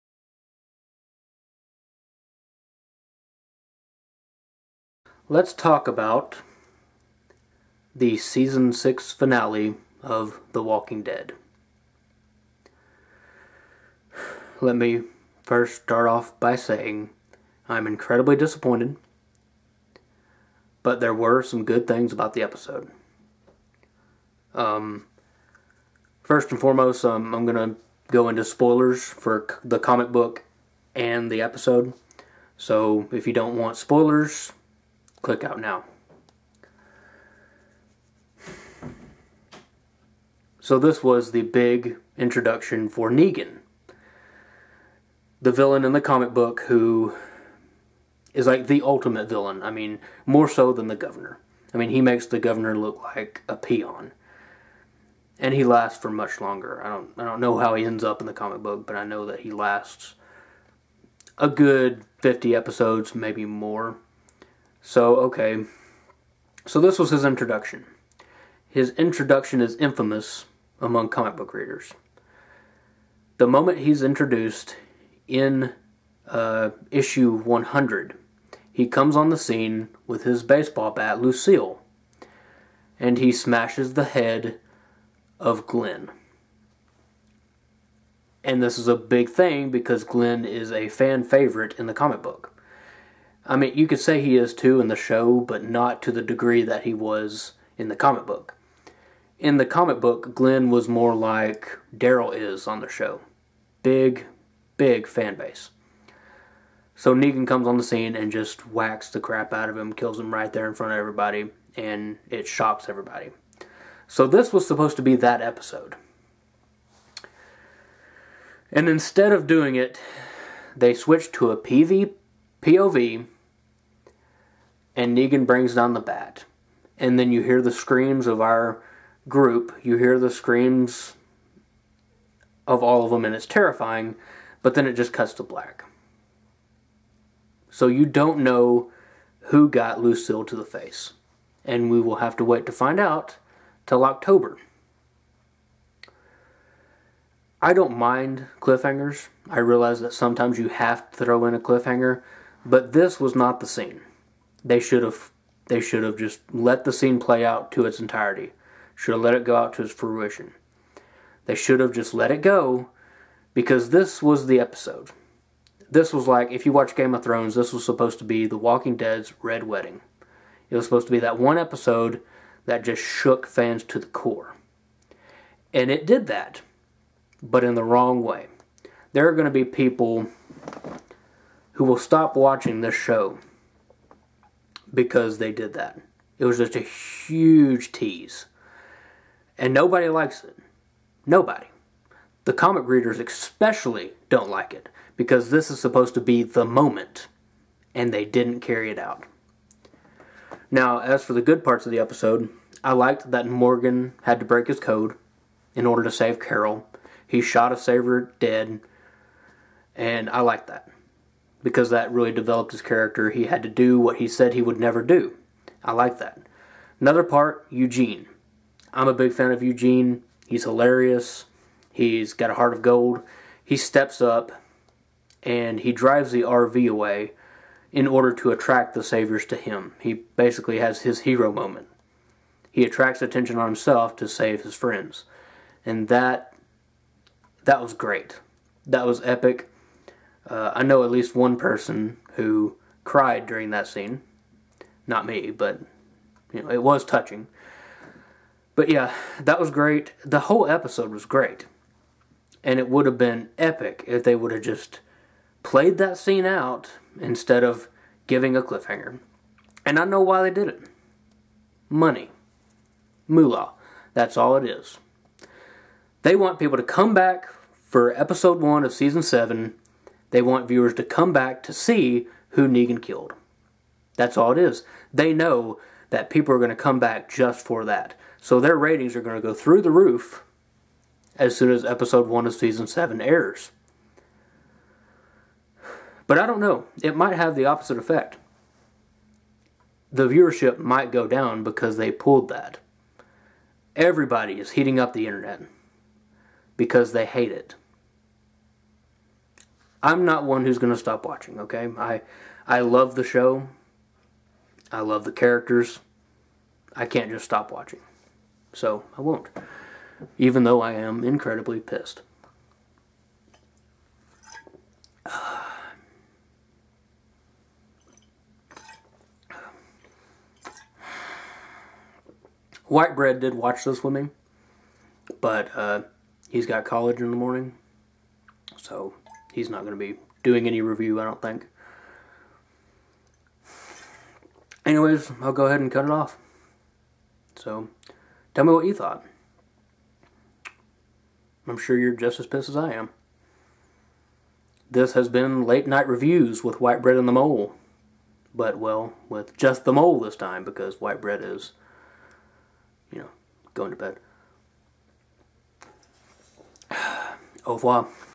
!!Spoilers!! The Walking Dead Season 6 Finale Review
The Mole reviews the season 6 finale of The Walking Dead and the introduction of Negan.